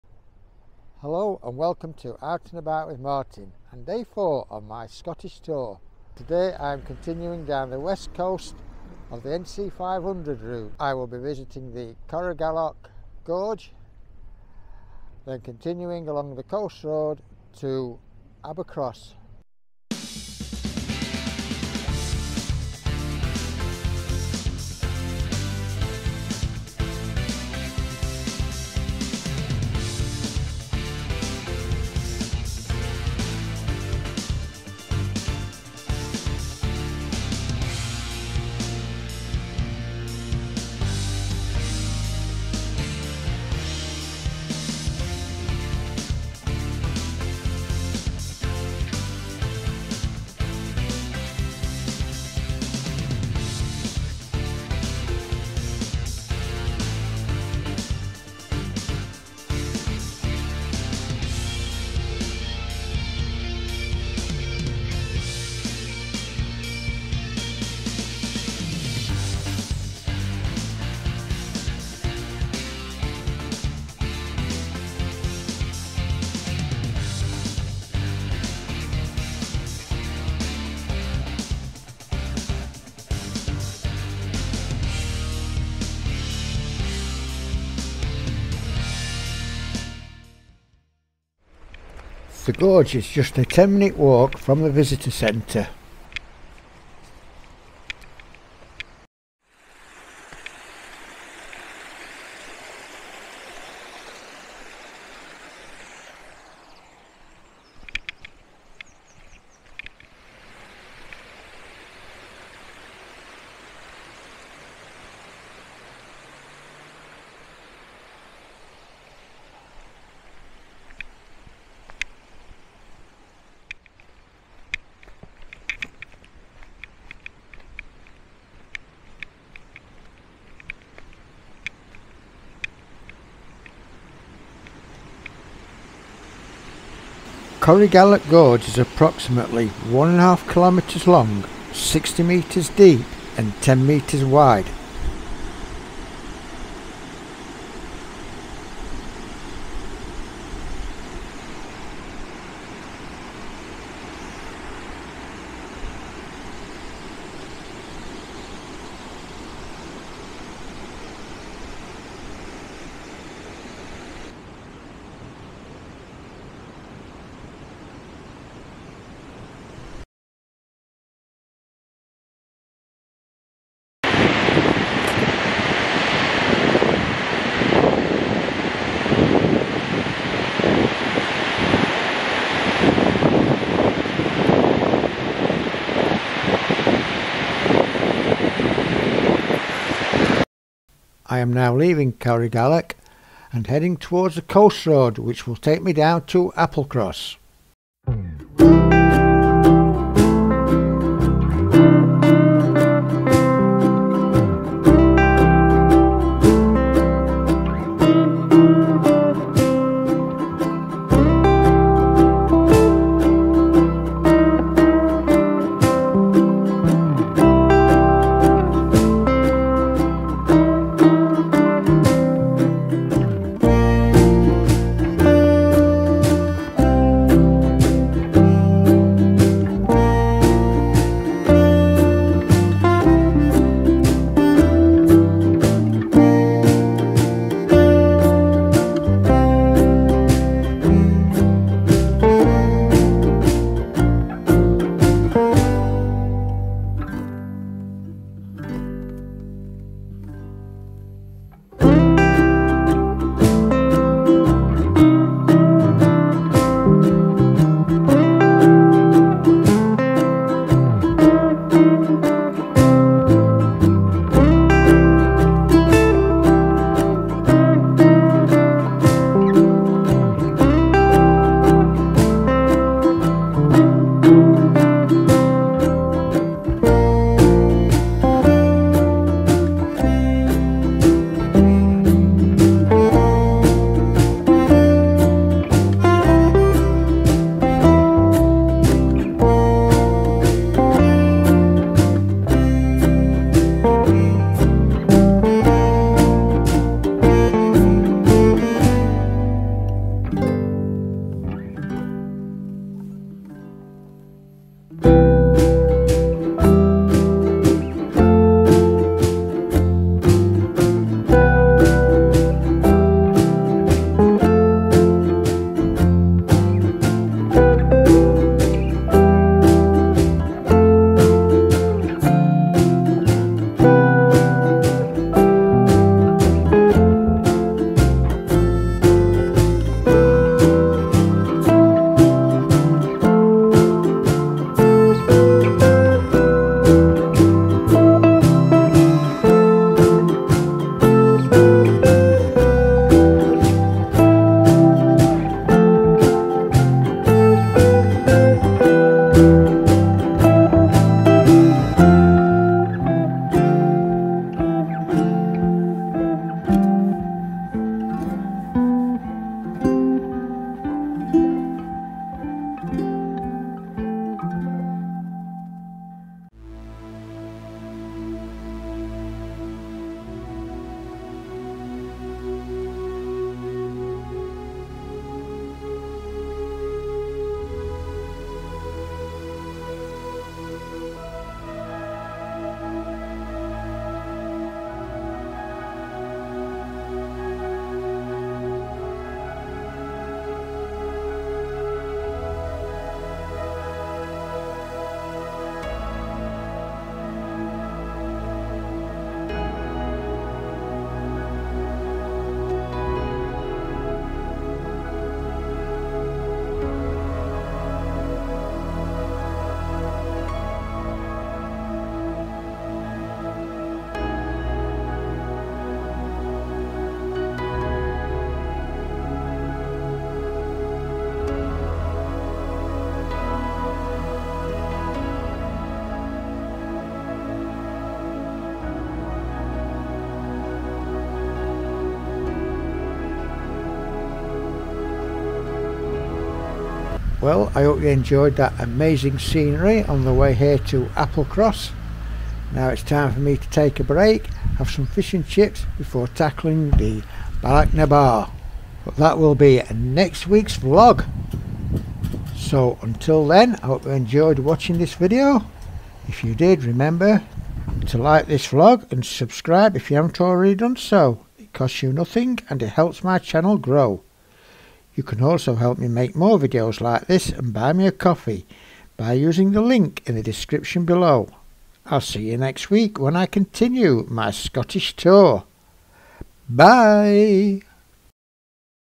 Day 4: Driving theNc500 route and Isle Of Skye. part 1
On day 4 I leave Ullapool and visit the nearby Corriegalloch Gorge befor continuing along the 100 mile section along the west coast to Applecross